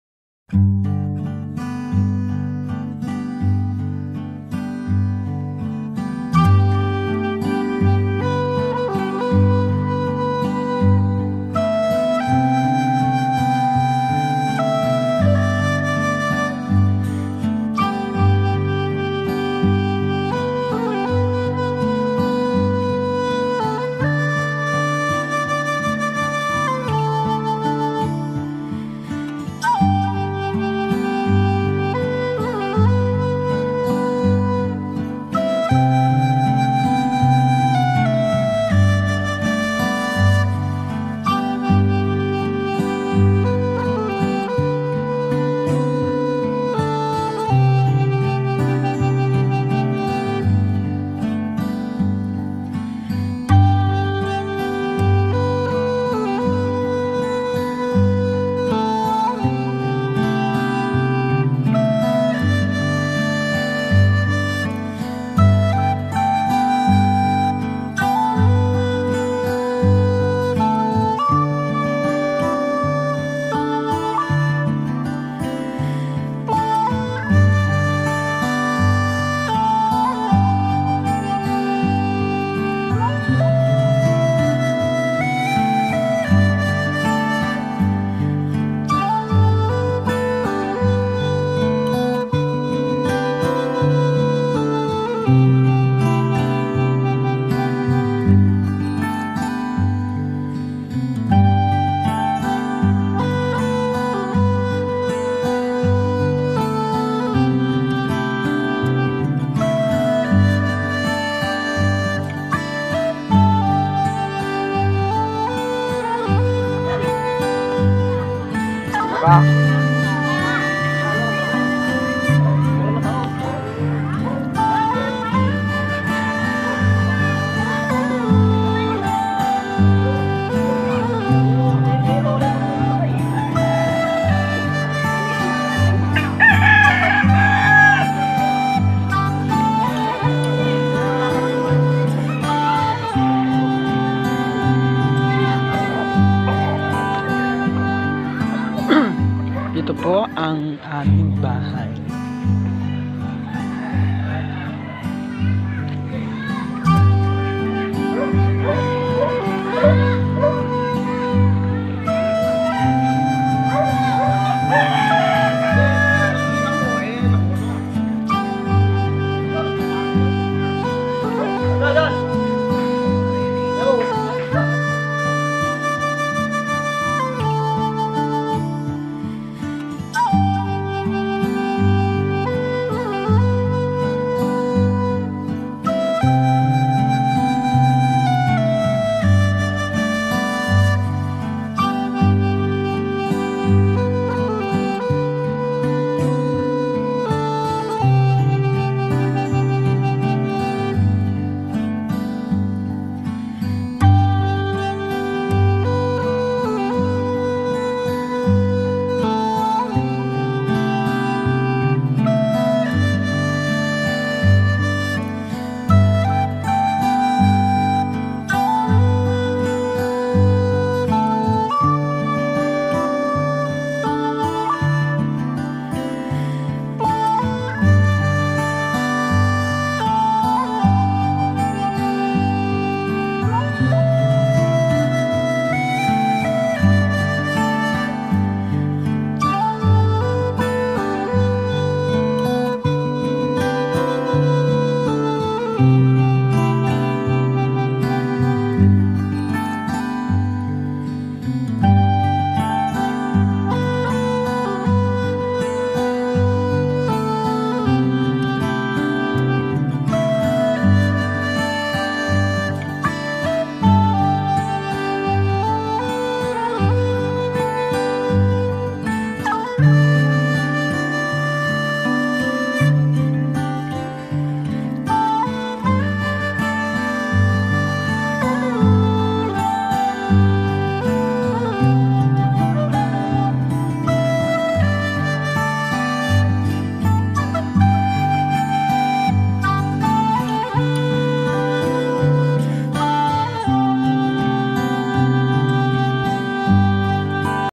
Typhoon Ulysses Aftermath
Pagkatapos ng bagyong Ulysses, nag iwan ito ng malaking pinsala. Mga buhay na nasawi, mga bahay na nasira, nawalan ng ikinabubuhay, at ngayon nahaharap po sila sa matinding kagipitan. Kapit lang mga kababayan,kaya nating bumangon. 🙏💪